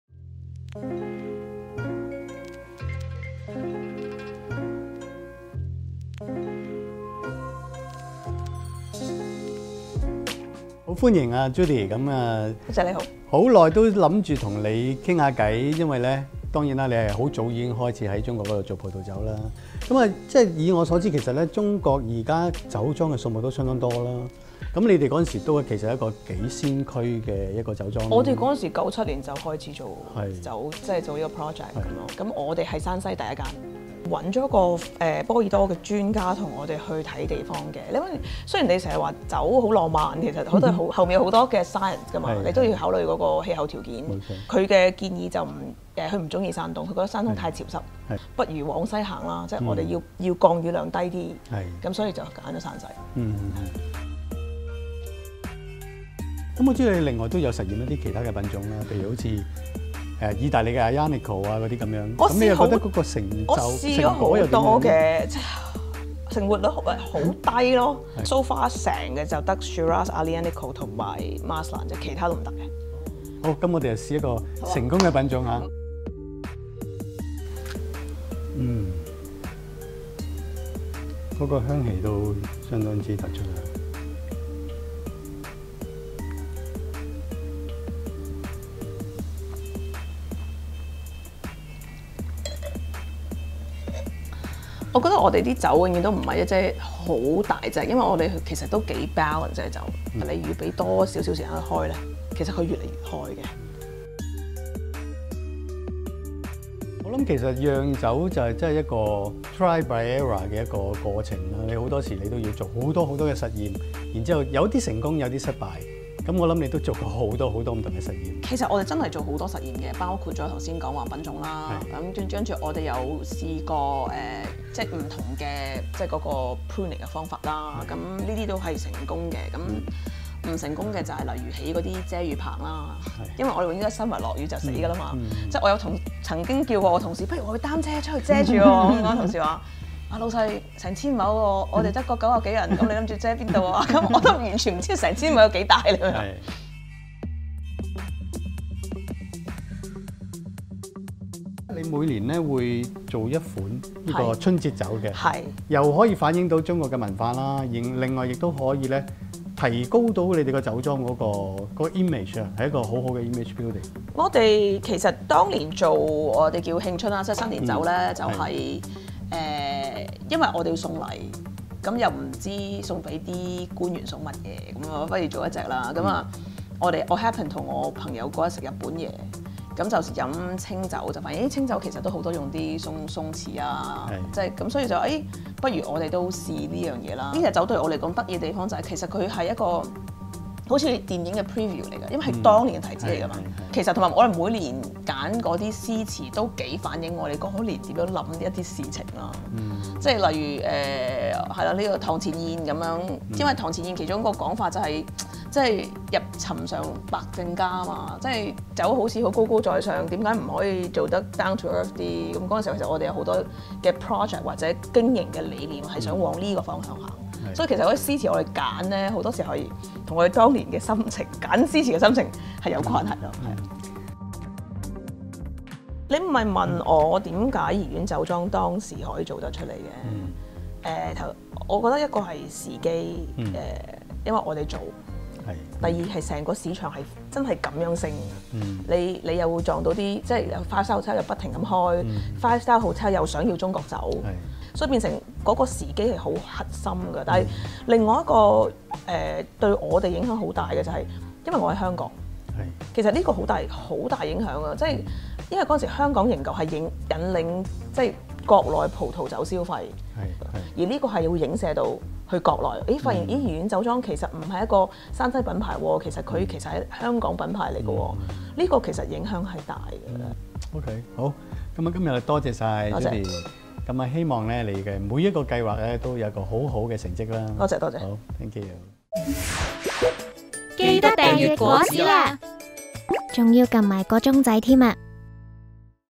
山西釀酒廿年趣事多 莊主陳芳：人手撐傘遮葡萄都諗過 酒精深度遊 籽想好食─果籽 香港 Apple Daily─原刊日期：20191020
Domaines Barons de Rothschild在中國山東省丘山投資酒莊釀酒，消息自2009年傳出，一直 只聞樓梯響，今年9月，瓏岱酒莊的葡萄酒終於正式宣佈面世，首個年份2017，將於11月底付 運。十年磨一劍，加上波爾多一級酒莊Château Lafite Rothschild的光環加持，立即令中國成 為了酒壇焦點。

其實進軍中國葡萄酒產業的外國勢力又豈止Lafite，法國MHD的Domaine Chandon和傲雲，還有Pernod Ricard的賀蘭山等等，均已經開始取得成績。不過，正當外資陸續進軍中國，國產的葡萄酒卻又在努力衝出中國，開拓海外市場。